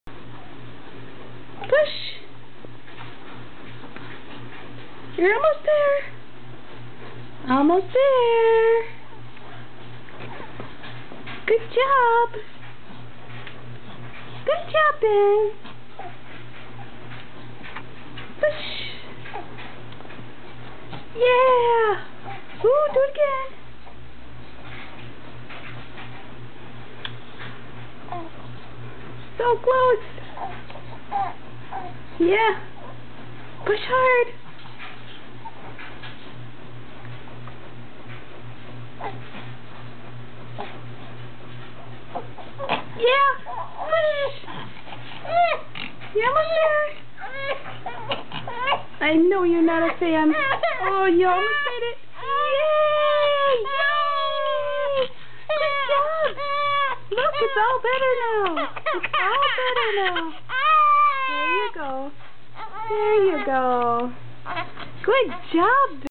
Baby Ben 15 Weeks Rolling Over
Ben hates tummy time with a passion, but has learned to roll over and we finally got a video of him accomplishing the roll.